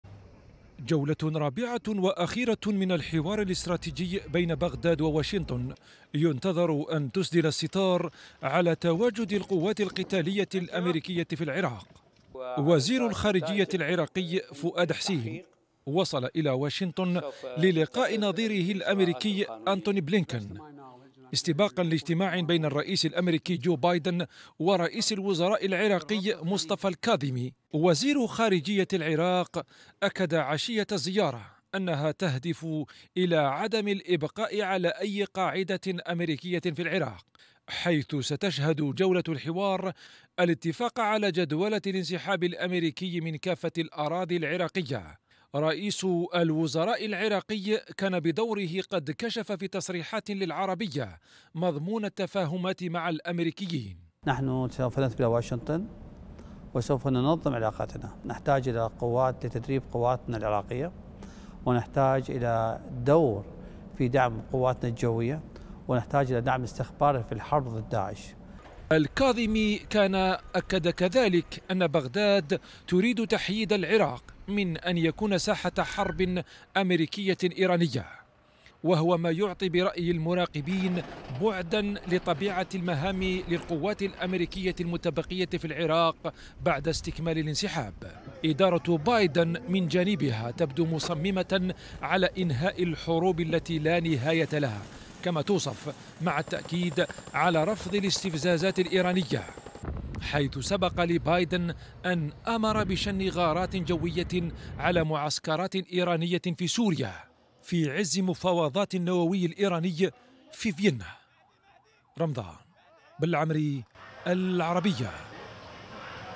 زيارة هامة ومصيرية لوزير خارجية العراق إلى واشنطن
قبل جولة رابعة وأخيرة من "الحوار الاستراتيجي" بين بغداد و واشنطن.. وزير خارجية العراق في واشنطن للتمهيد لمباحثات سحب القوات الأميركية من العراق